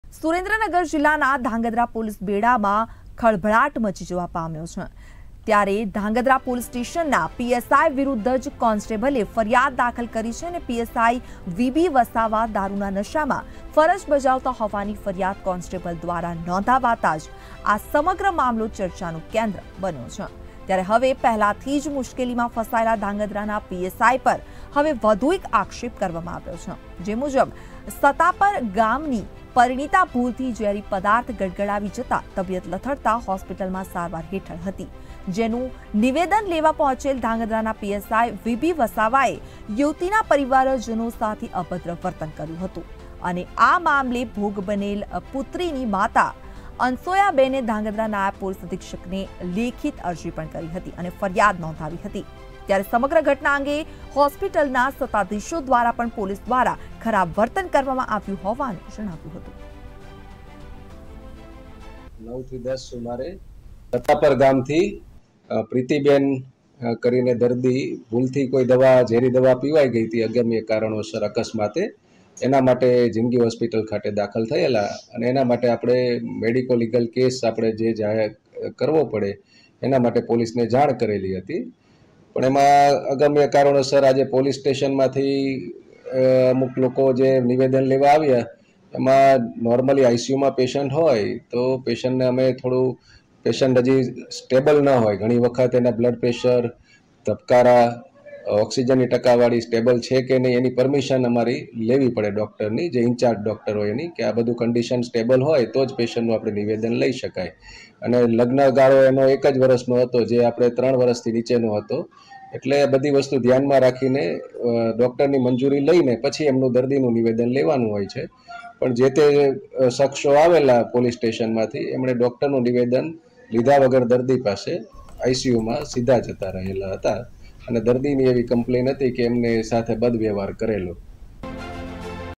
ધ્રાંગધ્રામાં PSI વીબી વસાવા વધુ એક વિવાદમાં
SURENDRANAGAR : ધ્રાંગધ્રામાં PSI વીબી વસાવા વધુ એક વિવાદમાં
નિવેદન લેવા પહોંચેલા PSIએ કર્યું અભદ્ર વર્તન
ભોગ બનનાર યુવતીના પરિજનોએ નોંધાવી ફરિયાદ